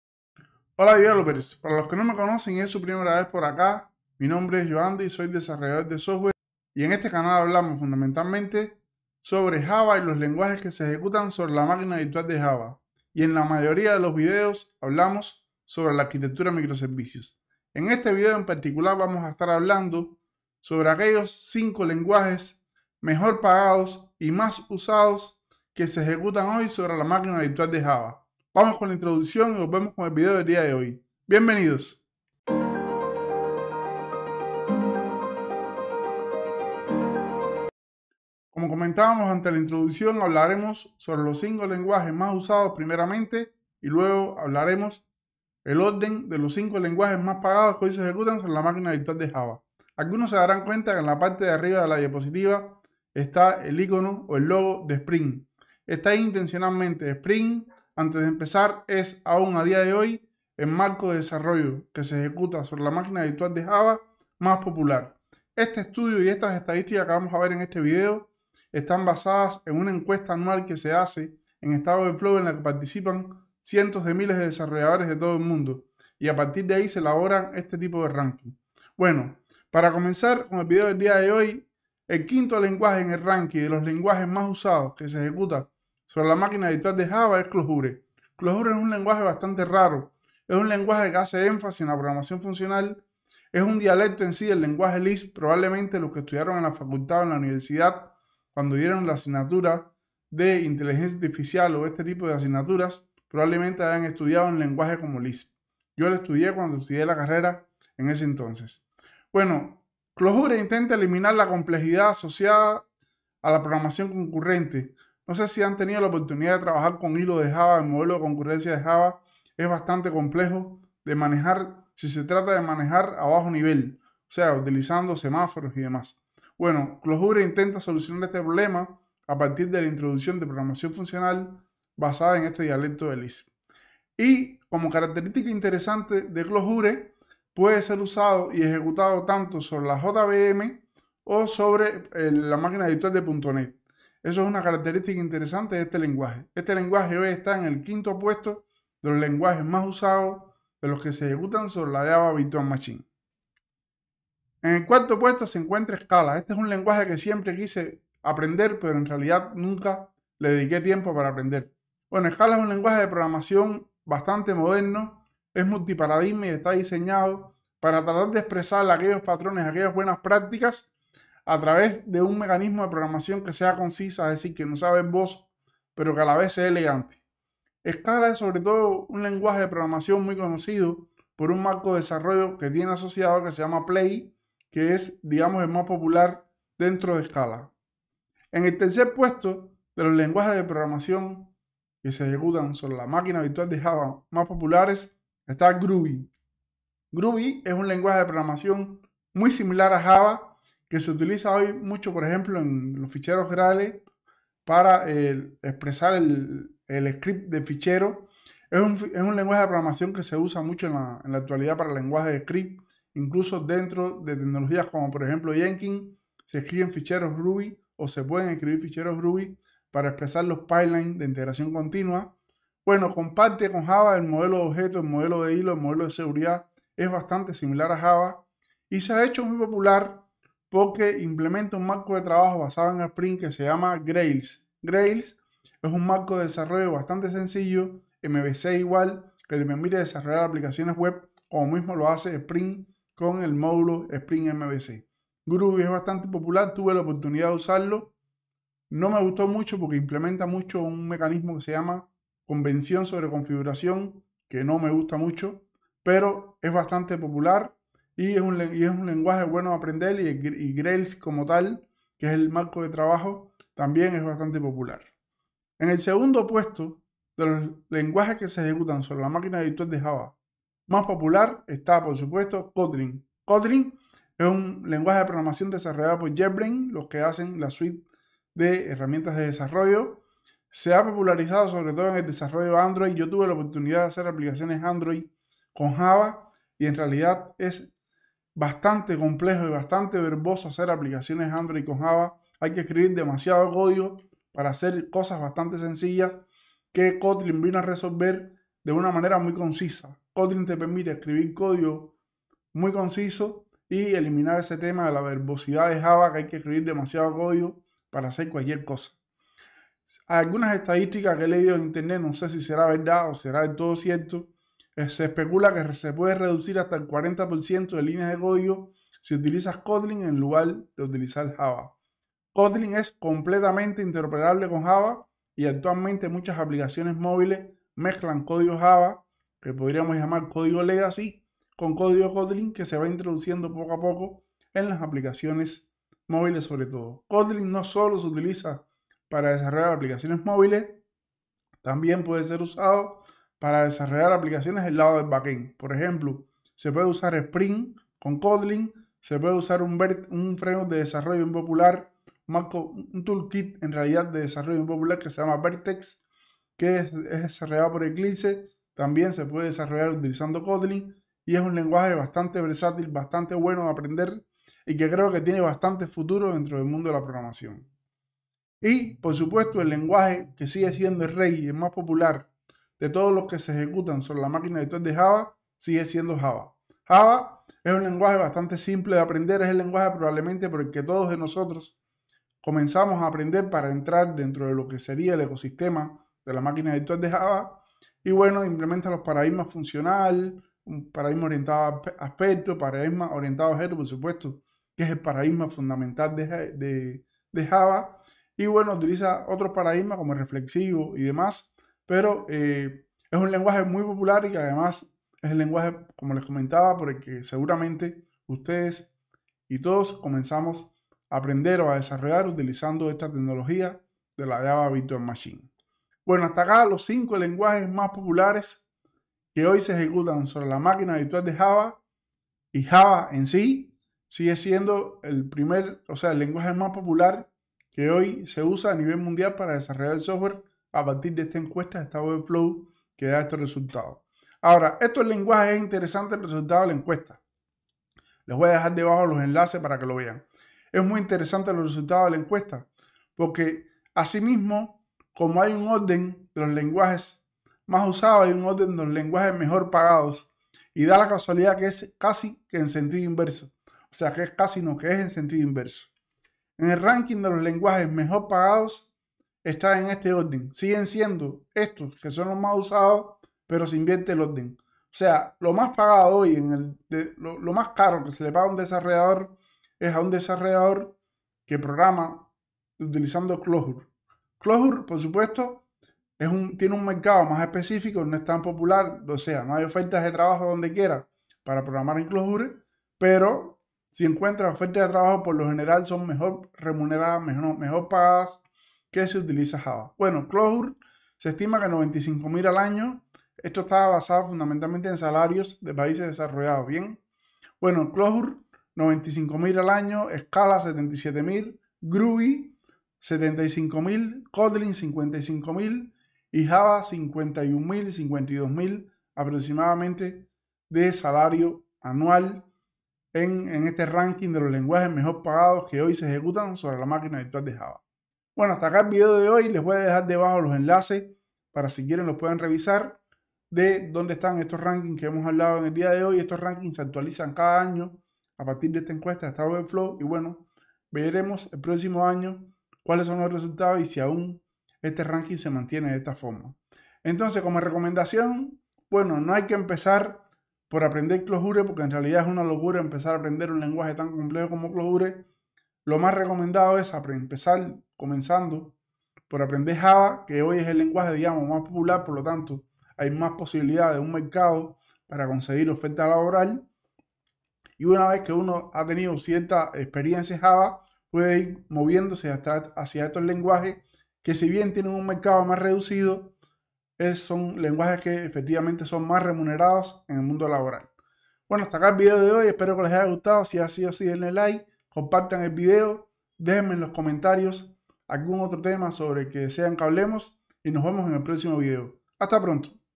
Top 5 lenguajes JVM más usados y mejor pagados
Muchos son los lenguajes que se ejecutan sobre la JVM, en este vídeo hablaremos sobre los 5 lenguajes más populares y mejor pagados.

Este vídeo está basado en la encuesta anual de stackoverflow.

Lenguajes mejor pagados
Clojure
Scala
Groovy
Kotlin
Java